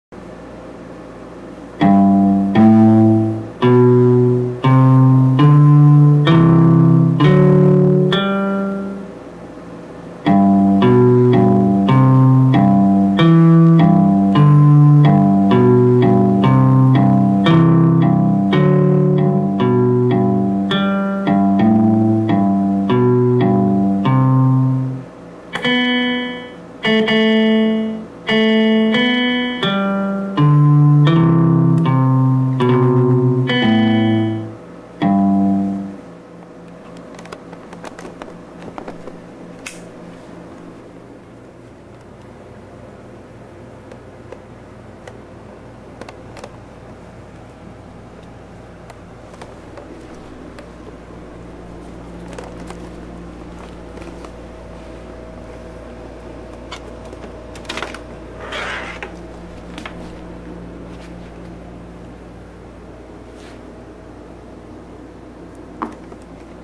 USB guitar plays Les Paul Google Doodle
In the true innovative spirit of Les Paul, this home-built, hand-wired, USB guitar plays the Les Paul Google Doodle guitar!